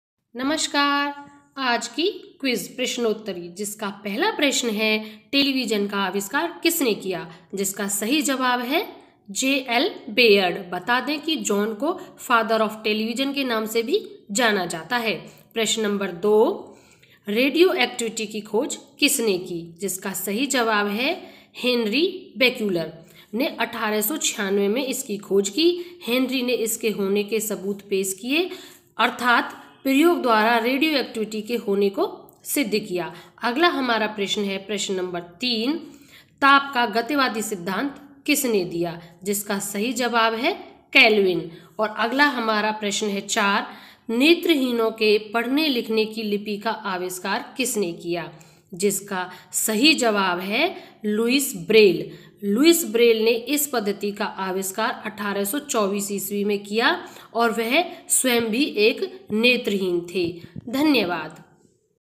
Quiz/ आओ जाने बच्चे कैसे प्रश्नों को याद रखना सीखते हैं।
बच्चों के सामान्य ज्ञान को बढ़ाने के लिए प्रतिदिन चार प्रश्नों का संकलन करना ज्यादातर आविष्कार को शामिल करना।